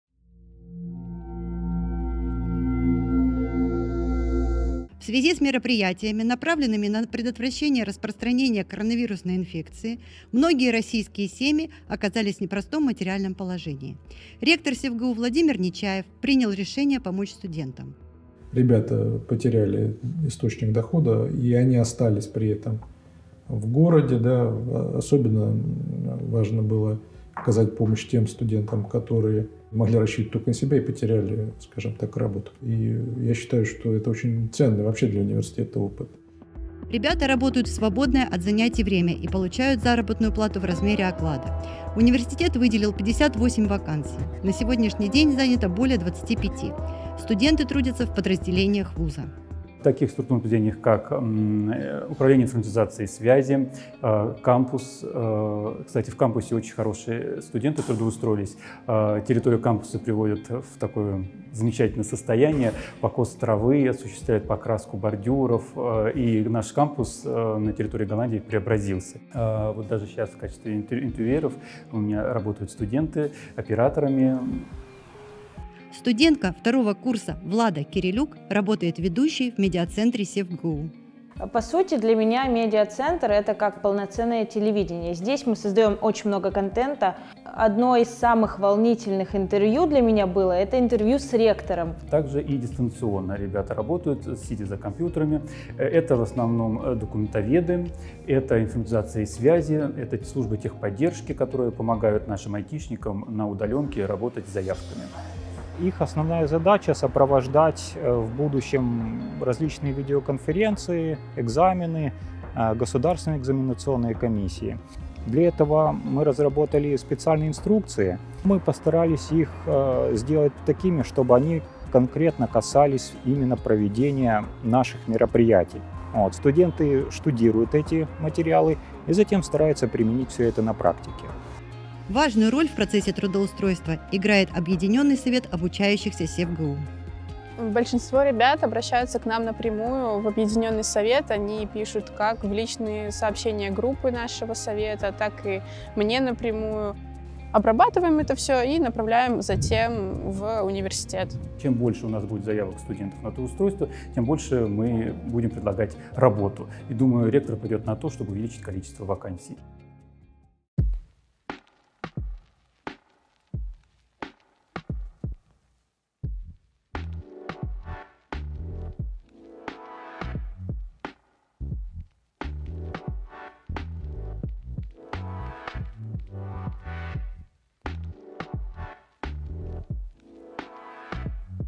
СевГУ берет на работу студентов
Университет выделил вакансии для студентов на время пандемии, чтобы поддержать их материально.

В связи с мероприятиями, направленными на предотвращение распространения коронавирусной инфекции, многие российские семьи оказались в непростом материальном положении. Ректор СевГУ Владимир Нечаев принял решение помочь студентам. В основном это малоимущие и малообеспеченные учащиеся, дети-сироты, инвалиды и остро нуждающиеся в подработке.

Ребята работают в свободное от занятий время и получают заработную плату в размере оклада. Университет выделил 58 вакансий. На сегодняшний день занято более двадцати пяти. Студенты трудятся в подразделениях вуза.